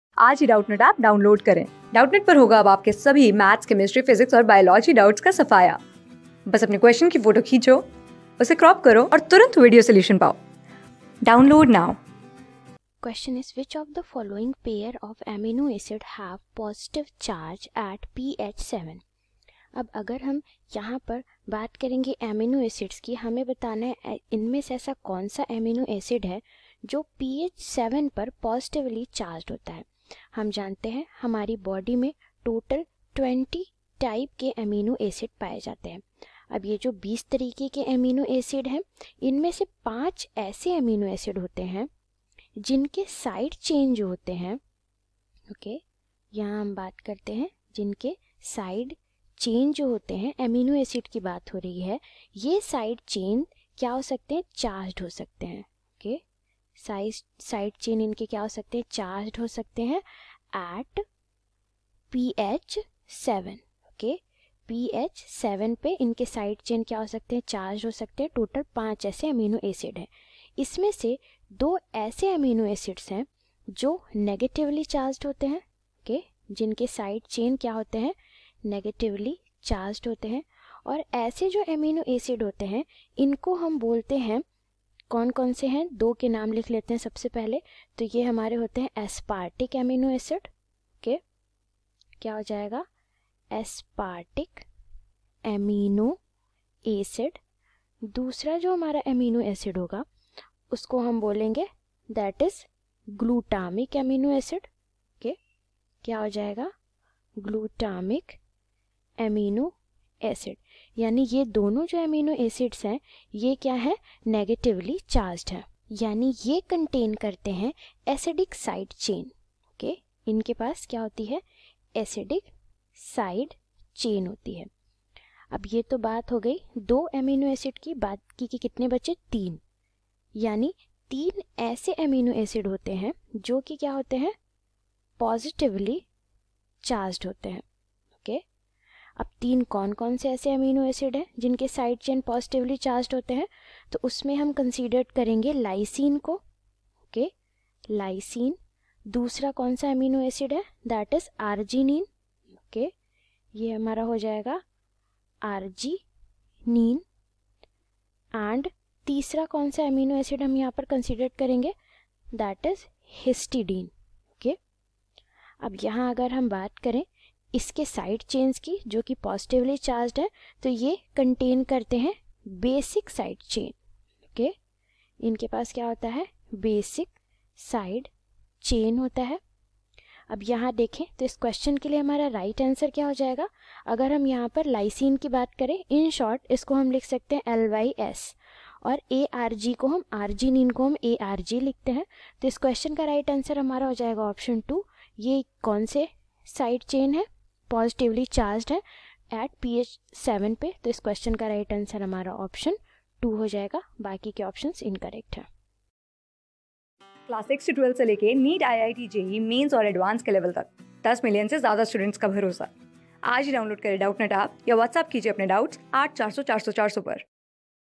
Which of the following pair of amino acid have positive charge at pH = 7 : | CLASS 12 | BIOMOLEC...
Which of the following pair of amino acid have positive charge at pH = 7 :
Class: 12
Subject: BIOLOGY
Chapter: BIOMOLECULES
Board:IIT JEE

👉 Have Any Query? Ask Us.